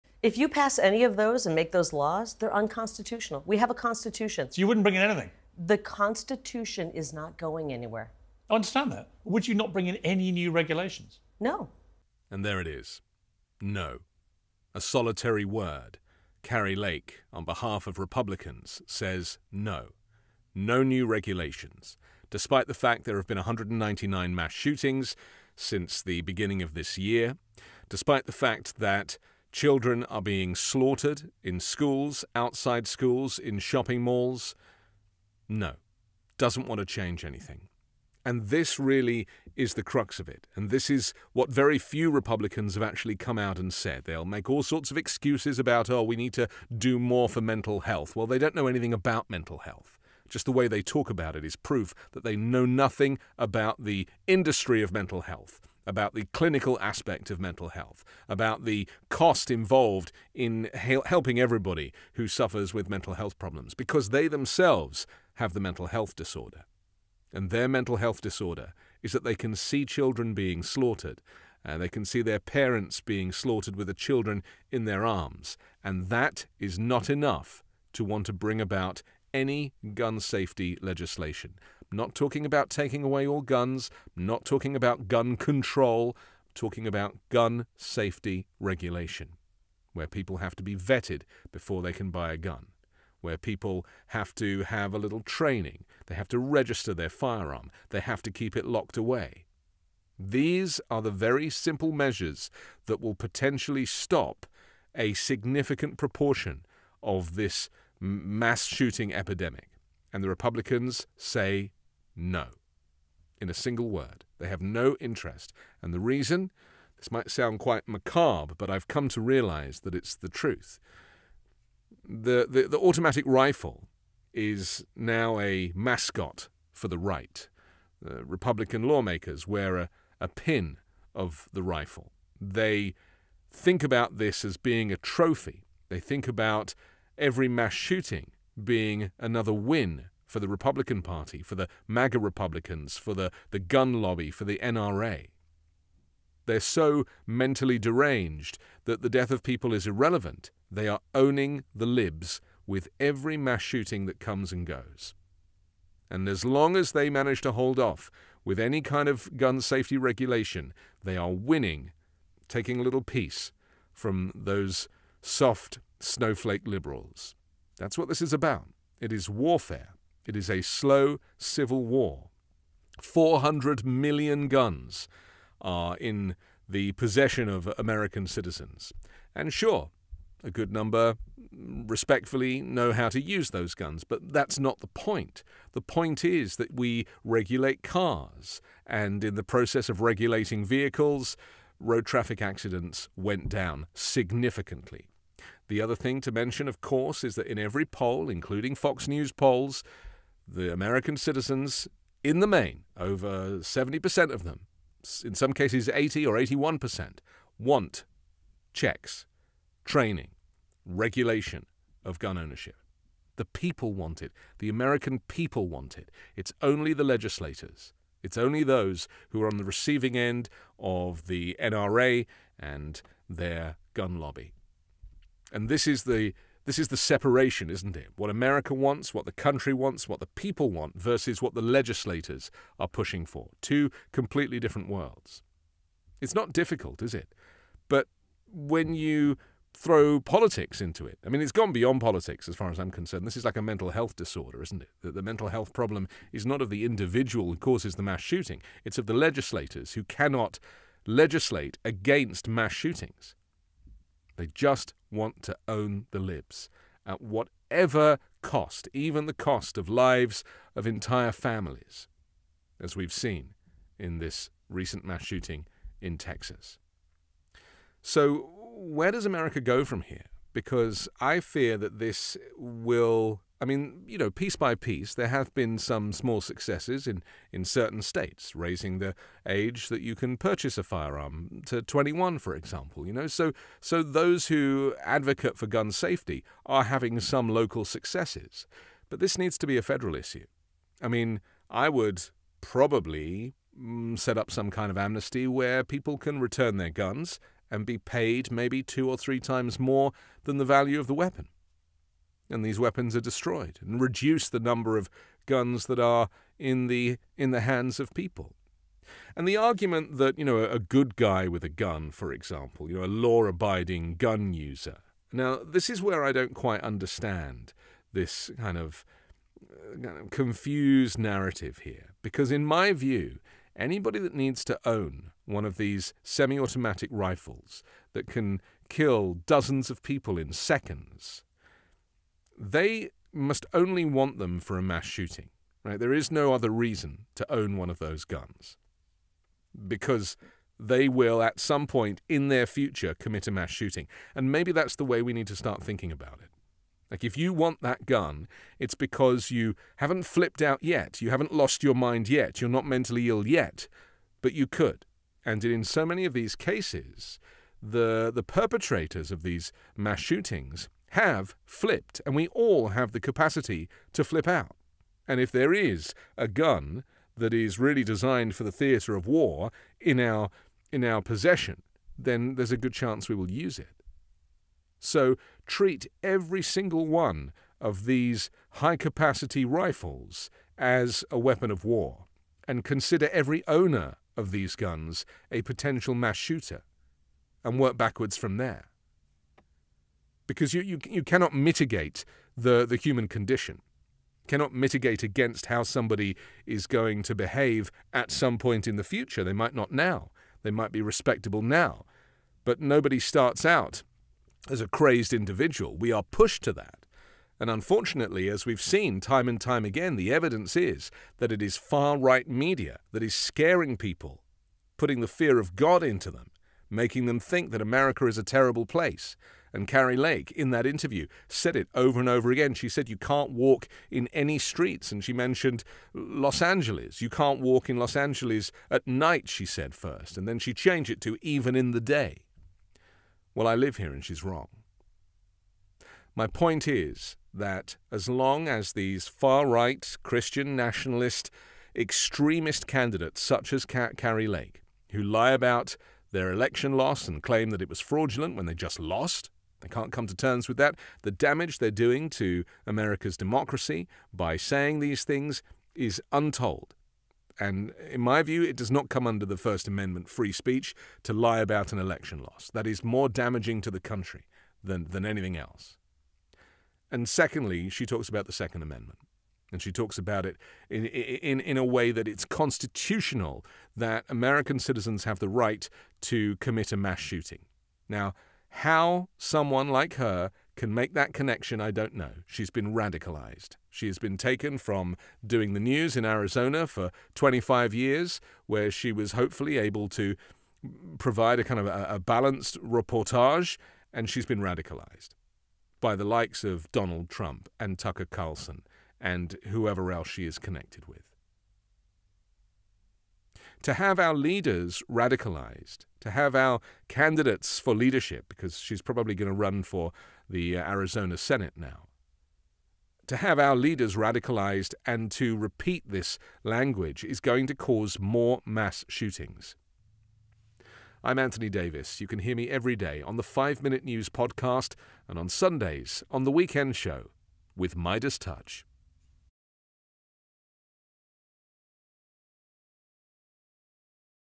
Kari Lake admits Republicans won't support gun safety legislation, no matter how many people die.
Anthony Davis explains the reason for AR-15 style weapons ownership and why Kari Lake won't support gun safety legislation, no matter how many people die.

Five Minute News with Anthony Davis is an Evergreen Podcast, covering politics, inequality, health and climate - delivering independent, unbiased and essential world news, daily.